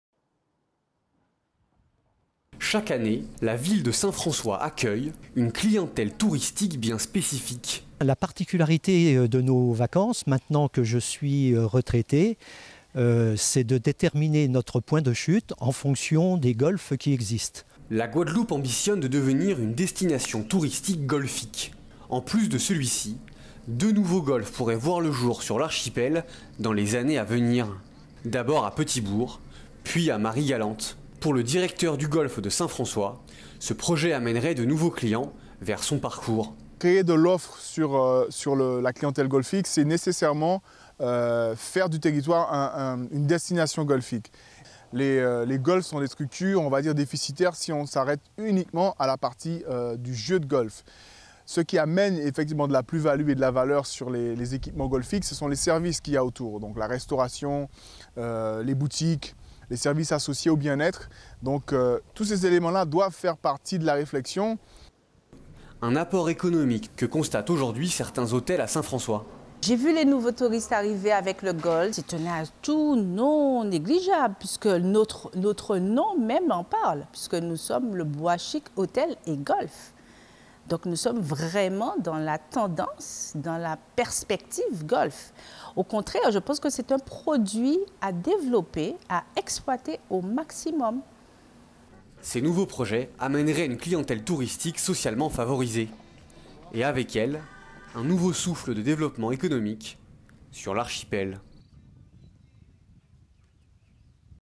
De nouveaux golfs en guadeloupe?
Avec la construction de deux nouveaux golfs, la Guadeloupe attirera bientôt un nouveau type de touristes.